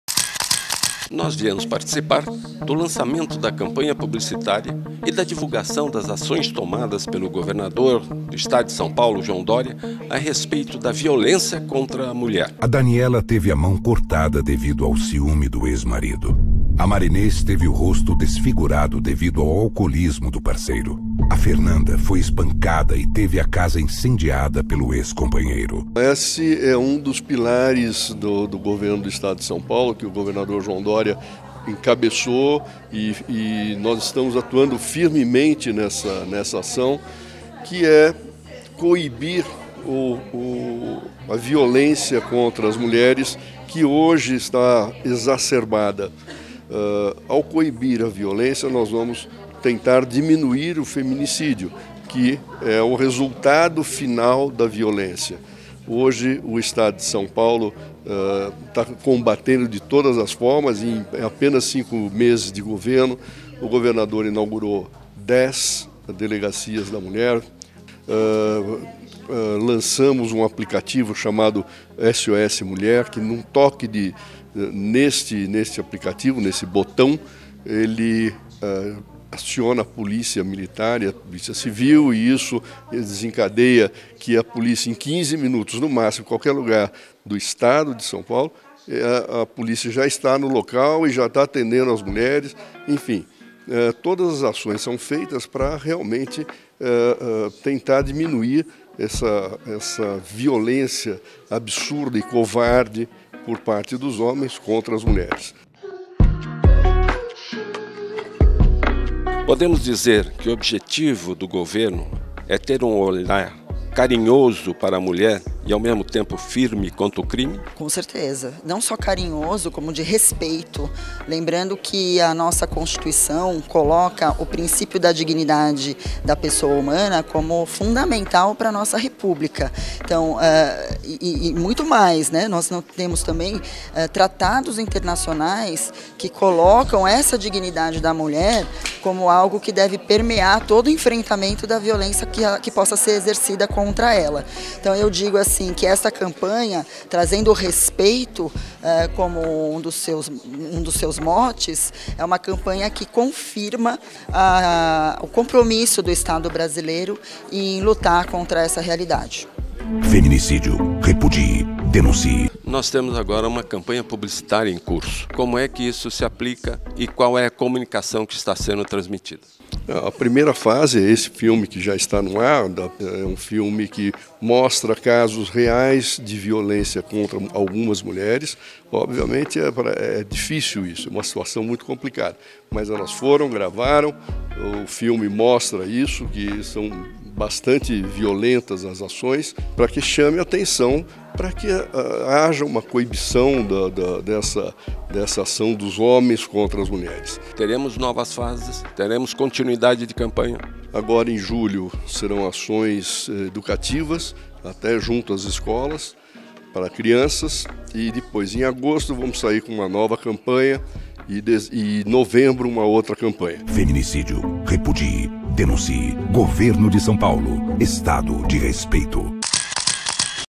Governo de São Paulo lança campanha em defesa da mulher
Grandes Nomes da Propaganda acompanhou o lançamento da campanha de combate ao feminicídio, do Governo de São Paulo. A comunicação quer despertar o engajamento da sociedade no combate à violência doméstica.

Acompanhe entrevistas com:

Duilio Malfatti Junior – Secretário de publicidade e promoção da Secretária Especial de Comunicação Social

Fabiola Sucasas – Promotora de Justiça do Estado de São Paulo

Edição: 22/2019